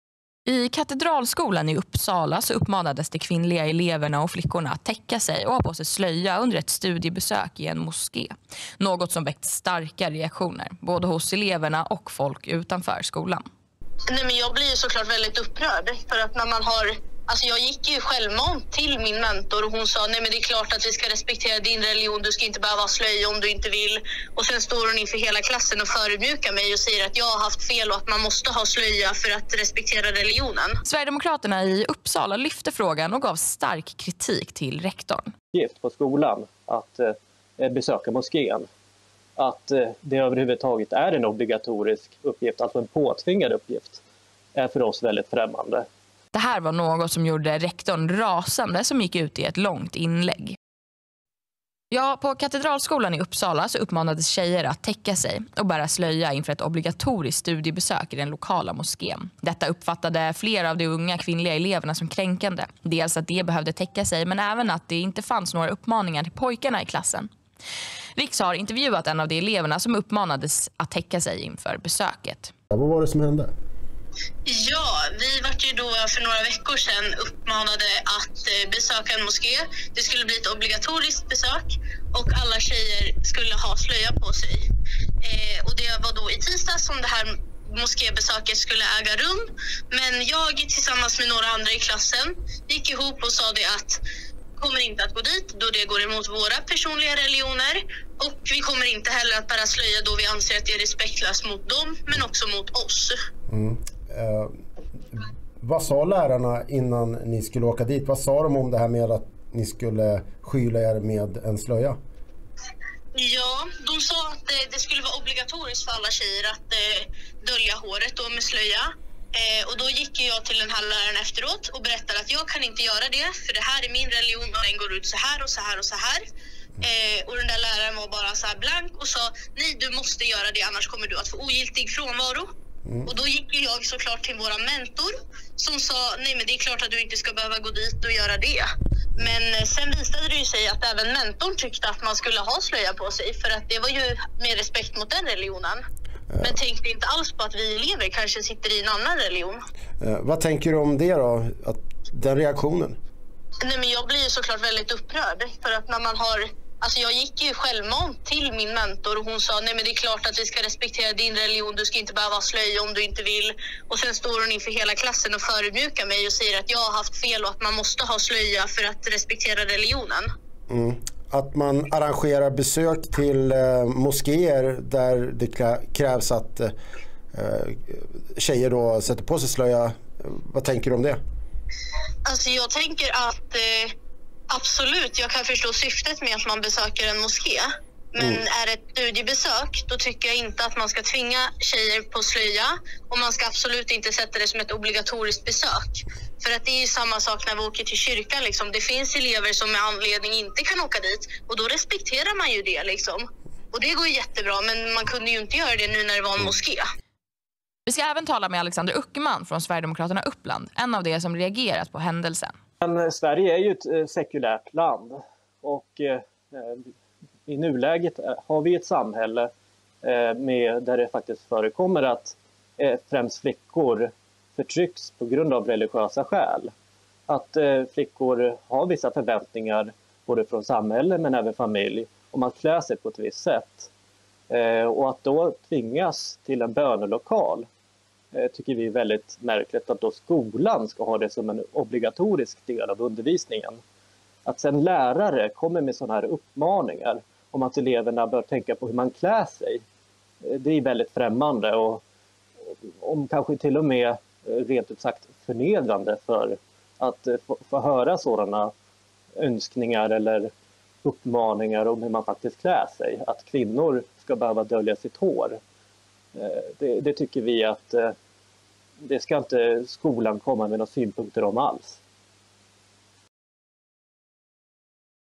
Skolans krav: Kvinnliga elever skulle bära slöja – elev: Jag vägrade
Inför ett studiebesök i Uppsala moské krävde lärarna på Katedralskolan i Uppsala att kvinnliga elever skulle bära slöja. Riks intervjuar en kvinnlig elev som vägrade att bära slöjan.